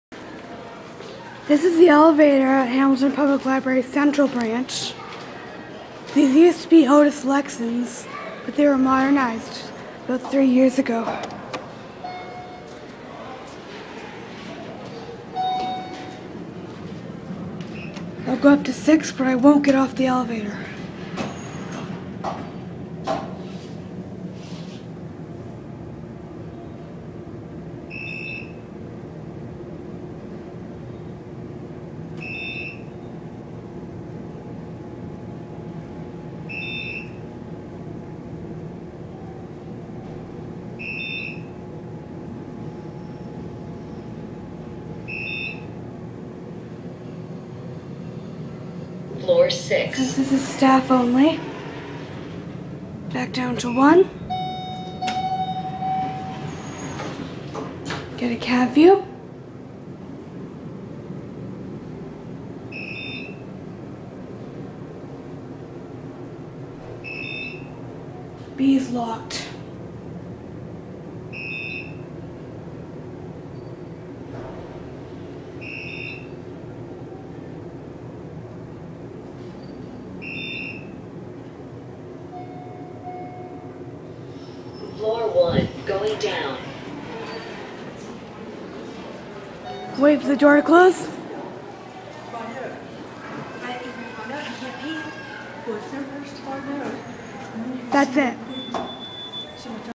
Unknown Elevator at Hamilton Central Library
These used to be Otis Lexans (with bright orange interiors), but they were modded and completely gutted about 3 years ago. I'm surprised they didn't break down after all the rides I took on them this afternoon! 5 and 6 used to be locked on these elevators, and B always has been. The first floor has changed A LOT since I was a kid; the other floors look the same as ever. Everything on the 4th floor has been moved down to the 3rd, and the 4th floor is now study space. The 6th floor is only accessible by elevator, and is staff only, which is why I didn't get off.